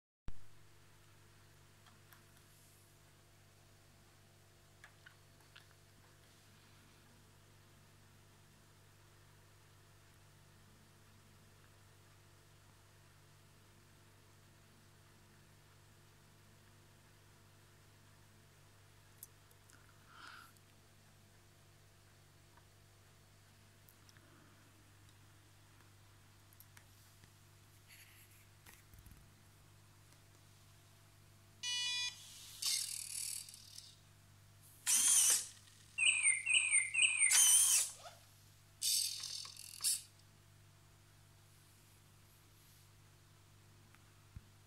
Ρολόι Κούκος, Cuckoo Clock - Gigo S4A Programming Bricks
Built with Gigo #1204 S4A Programming Block kit.
Programmed in S4A 1.6 , a Scratch modification (block programming) for easy programming of the Arduino open source hardware platform.